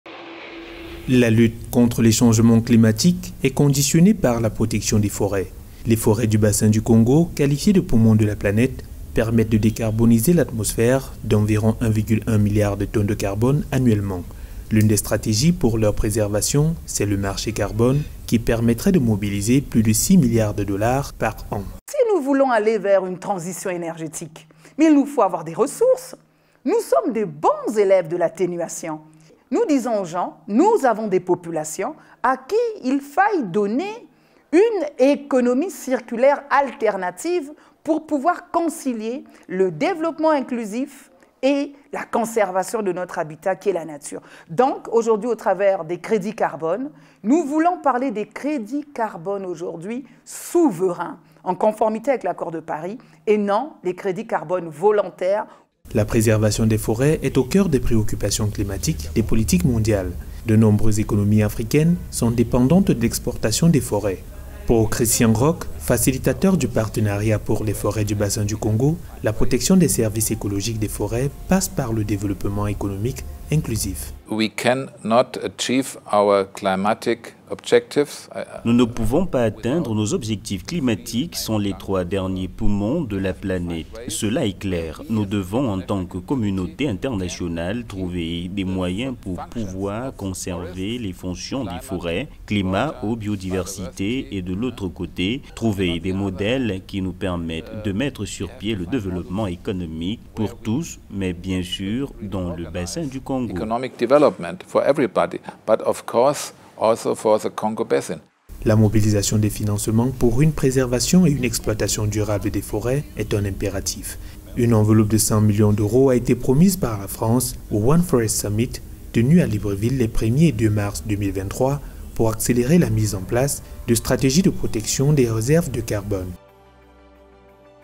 Afrique : le marché carbone, une opportunité pour les forêts
Environnement, Les experts des questions climatiques insistent sur la nécessité de préserver les forêts, moyen le plus efficace de prévenir l’avancée du réchauffement climatique. La préservation des forêts africaines passe par le développement de stratégies parmi lesquelles le développement du marché carbone qui permettrait au continent de mobiliser plus de 6 milliards de dollars annuellement pour son progrès économique.